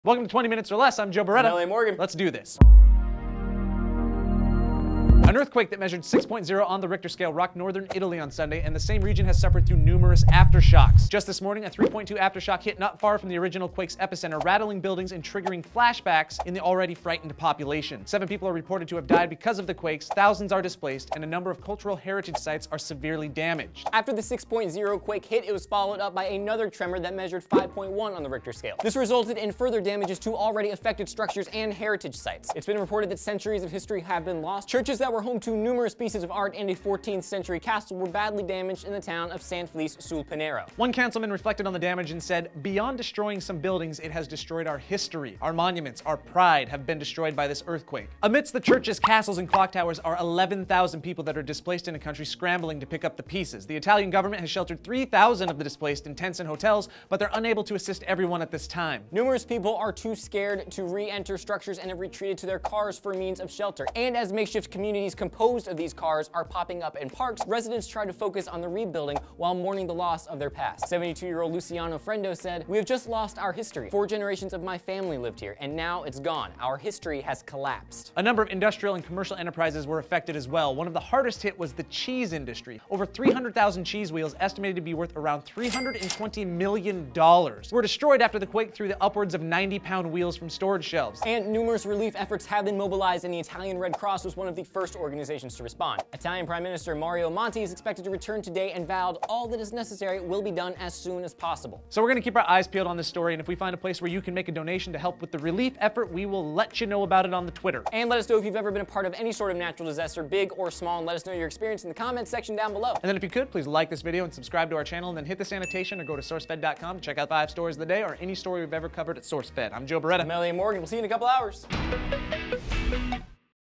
6.0 Earthquake Hits Italy
Earthquake in northern Italy has claimed the lives of six people and has caused an massive amount of damage.